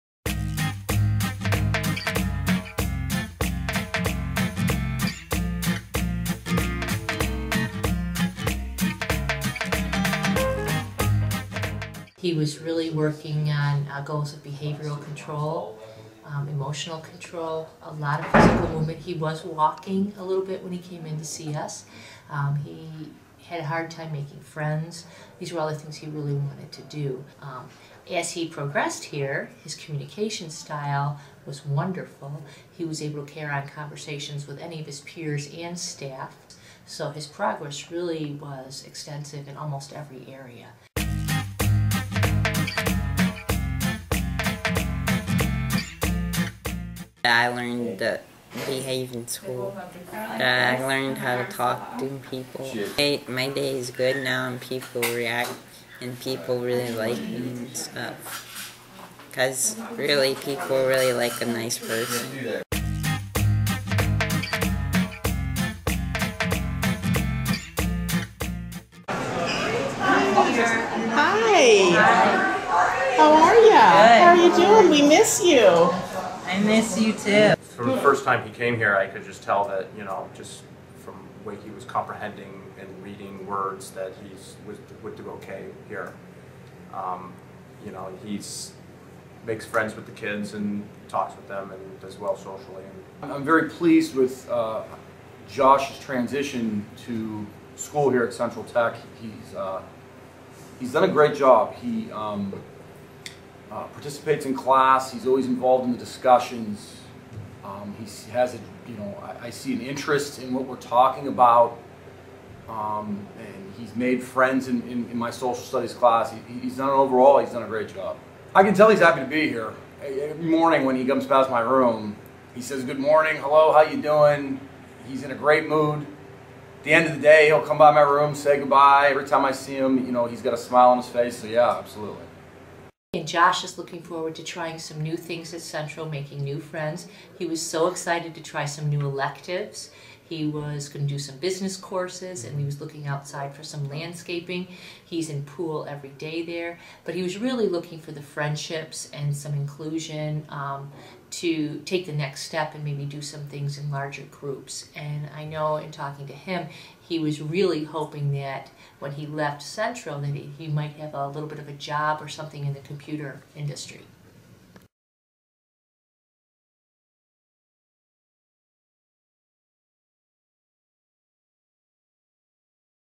A Dream Come True: High Hopes for High School with Josh
Follow along with Josh and his dream to attend his hometown high school!